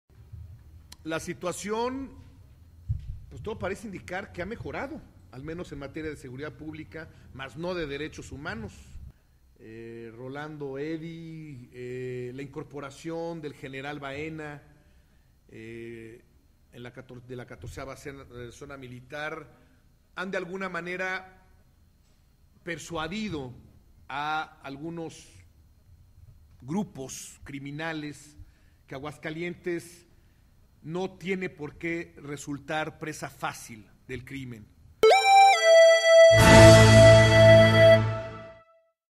Mejora de la seg
Aguascalientes - Mejora la seguridad en el estado pero la atenci&oacute;n a los derechos humanos, as&iacute; lo declaro el Pte. De las organizaciones no gubernamentales Martin Barberena Cruz.